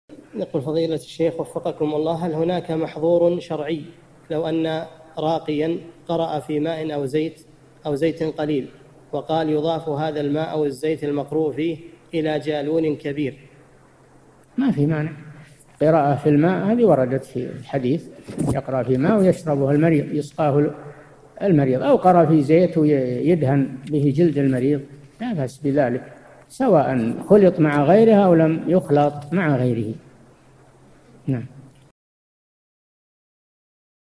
[1042 /1350] ما الرأي في راقٍ قرأ في ماء أو زيت قليل، وقال: يضاف هذا الماء أو الزيت إلى جالون كبير؟
-[1042 /1350] السؤال: هل هناك محظور شرعي لو أن راقيًا قرأ في ماءٍ أو زيتٍ قليلٍ، وقال: يضاف هذا الماء أو الزيت المقروء فيه إلى جالونٍ كبيرٍ؟
الجواب: ما فيه مانع، القراءة في الماء هٰذِه وردت في الحديث، يقرأ في ماء ويشربه المريض، يُسقاه المريض، أو قرأ في زيت ويدهن به جلد المريض، لا بأس بذلك، سواء خُلط مع غيره أو لم يُخلط مع غيره.
للاشتراك في قناة الشيخ صالح الفوزان
أسئلة وأجوبة من شرح قاعدة جليلة ( 1350 ) مقطع

للاستفسار والإقتراحات والدعم
رئيس الفريق
لمشروع كبار العلماء